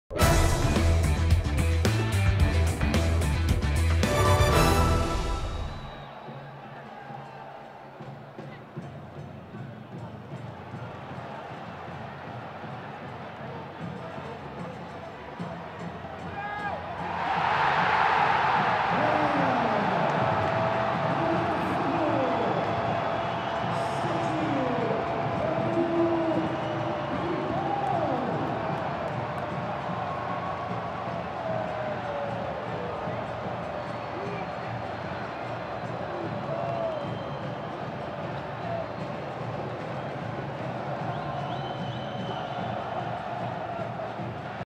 Botafogo sofre, mas Rodrigo Pimpão decide em vitória sobre o Bangu
Cliente: UOL
Serviço: Captação de imagens e edição
Produção: Léo Nunes
Realização: NNS FILMES ®